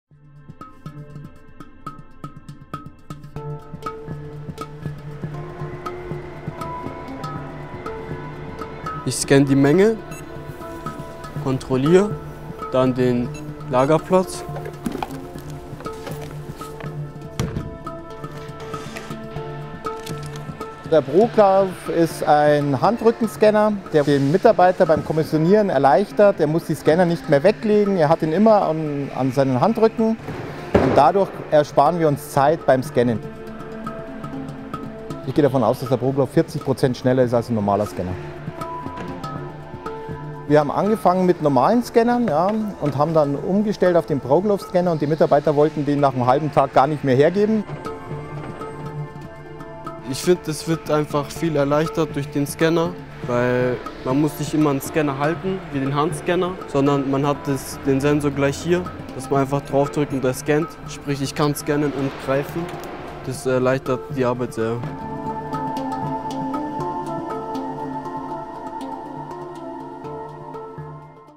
DB Schenker Germany - Implementing ProGlove
An intelligent scanning glove - ProGlove significantly improves process efficiency. While traditional ring scanners are not ergonomic and difficult to use, ProGlove provides an easy way of activating the scanner. The warehouse workers can continuously deploy both hands during operations without having to search or reach out to a scanner in between. In turn, this results in process savings of up to 40% in comparison to usual scanners.

In Eching, near Munich, DB Schenker supplies the production of a leading German car manufacturer with automotive parts. Services include unloading and physical collection of the goods, warehouse management, goods issue processing, empties handling incl. shipping, repacking of disposable packaging in reusable packaging and storage of automotive parts in large load carriers or small load carriers. The ProGlove will be used for order picking by scanning the barcodes of the automotive parts.